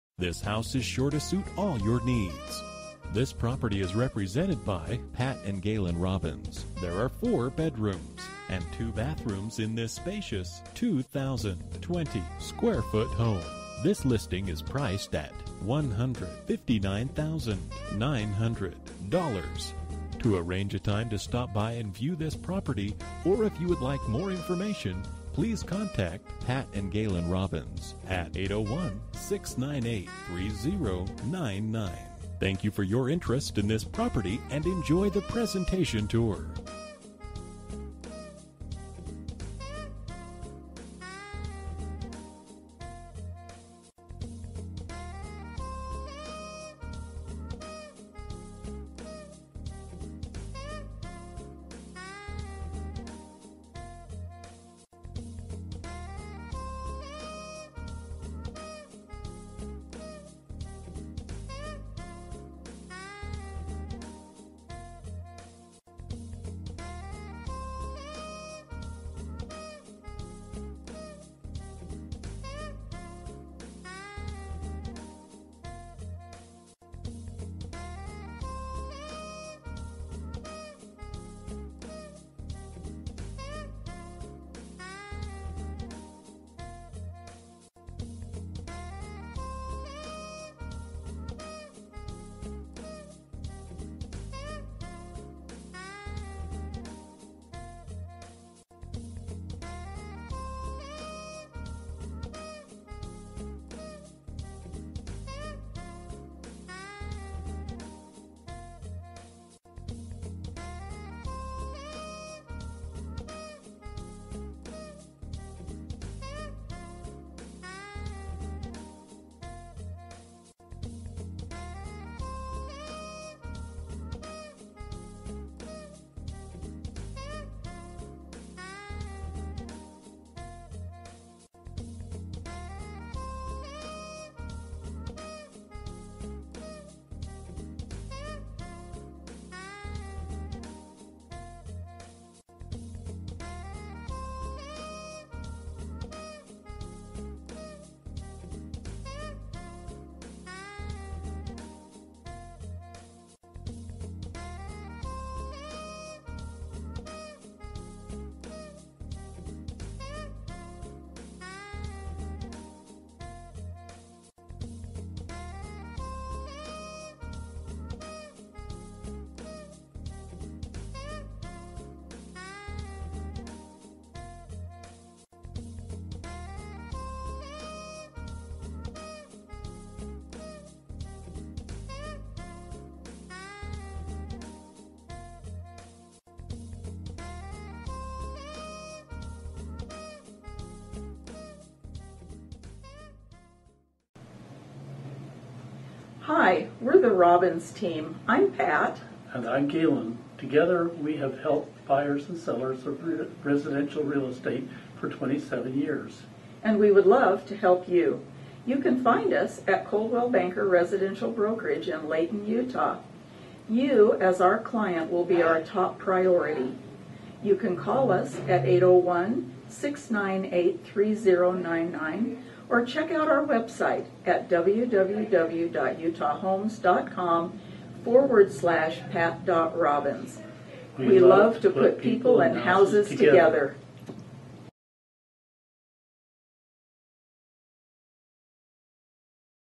1394 Marilyn Dr
Bedrooms: 4
Bathrooms: 2 bathrooms
Square Feet: 2020
Price: $159,900

For more information about this property, please contact Pat and Gailon Robins at or . You can also text 766498 to 67299.